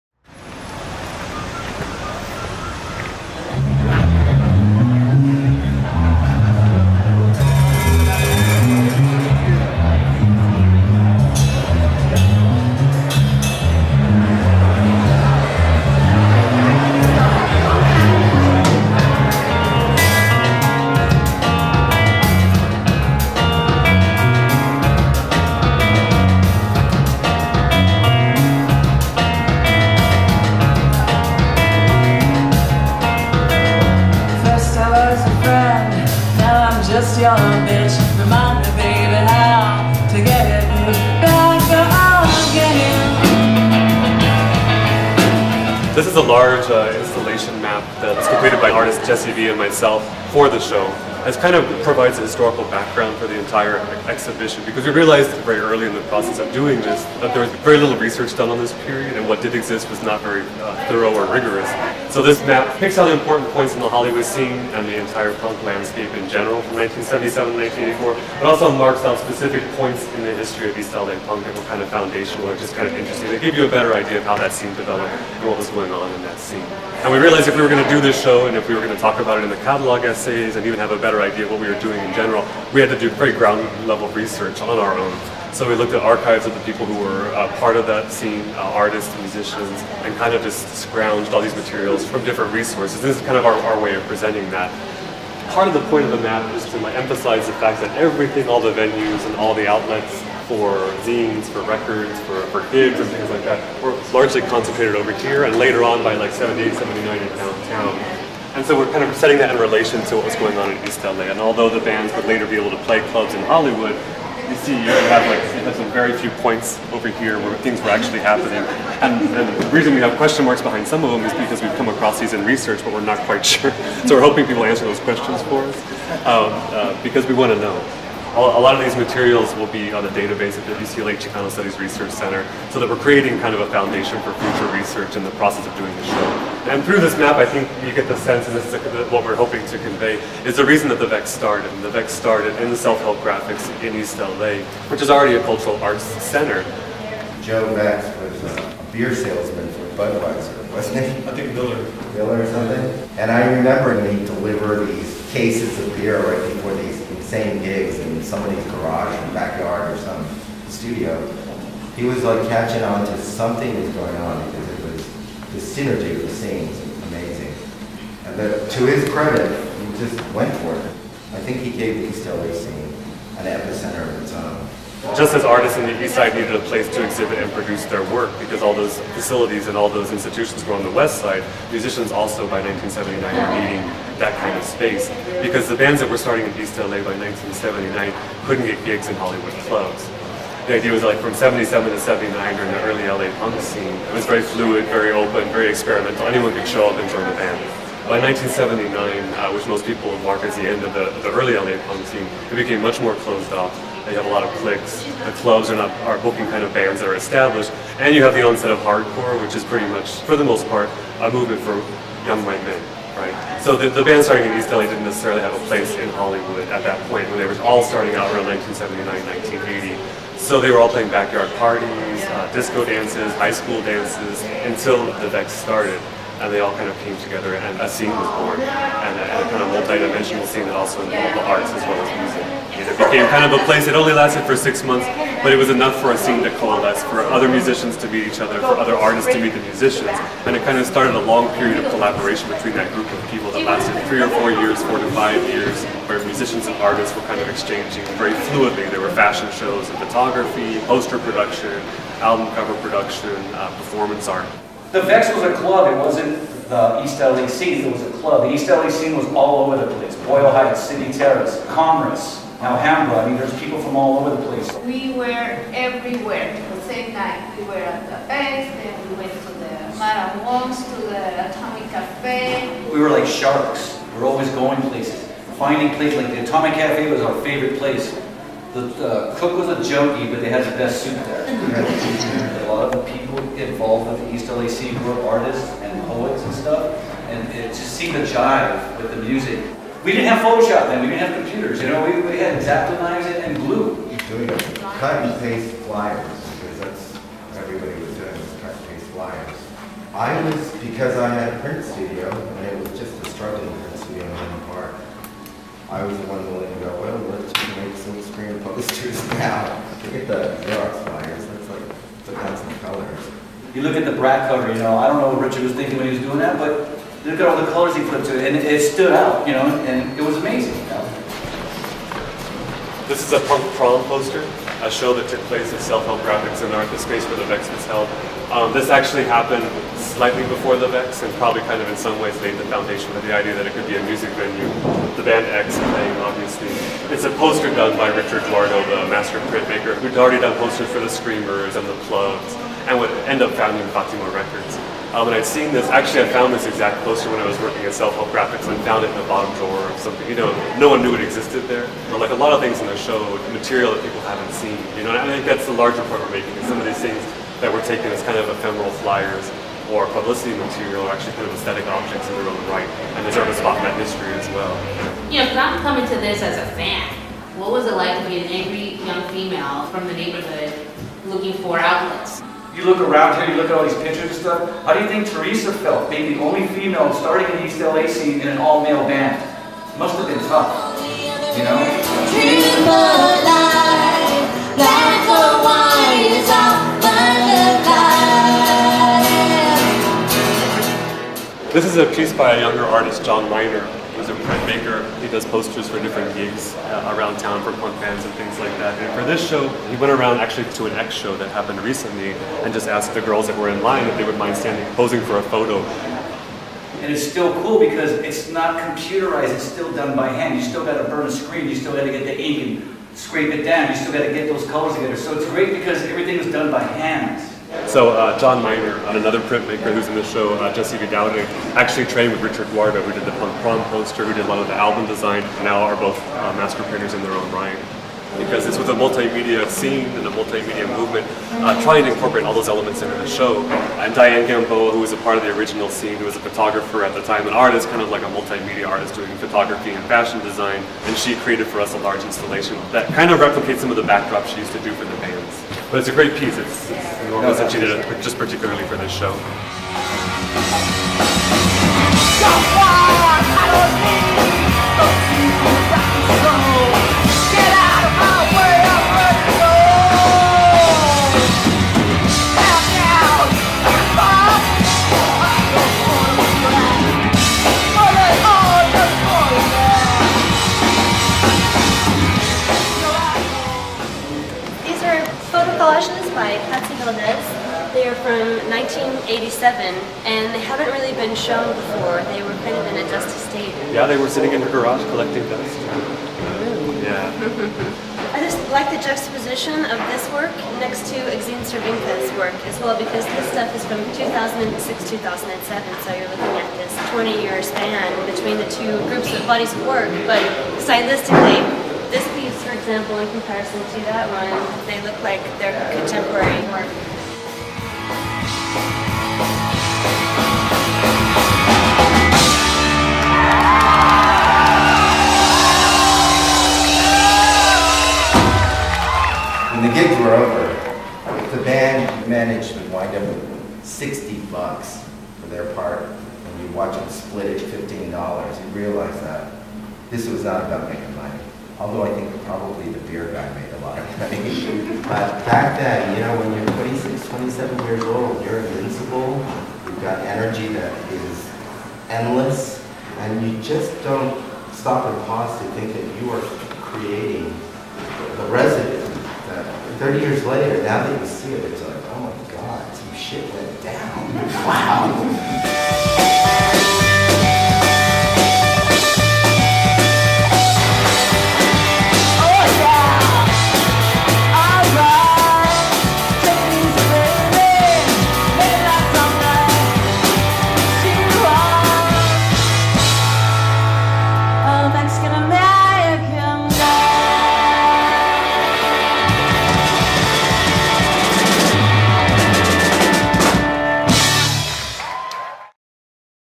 VEXING: Female Voices of East L.A. Punk
Co-curators Colin Gunckel and Pilar Tompkins present an inspiring array of materials from a lost chapter in punk music history.  This well-received exhibit was presented at the Claremont Museum of Art from May 18 to August 31, 2008.   An extra special thanks to the Museum staff, who granted access to videographers Steve Cioffi and Parris Patton.